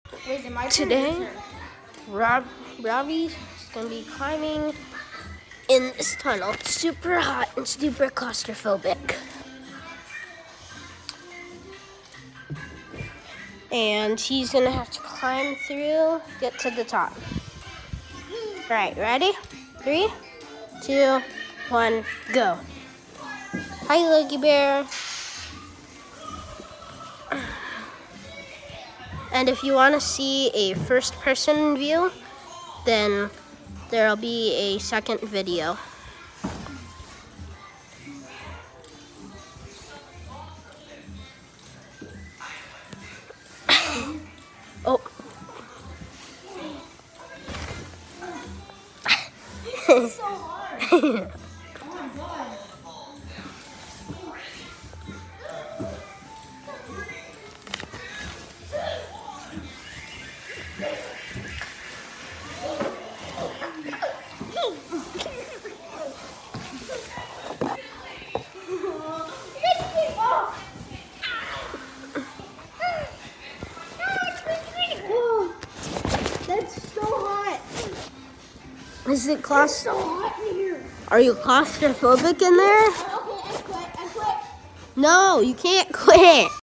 Trying to climb up a tunnel covered in blankets and pillows
I told my freind climb up a tunnel and try to make it to the top which is extremely hot 🥵 and make sure to sub to me if u want and sorry there is no 1st person view we tried but it was to hard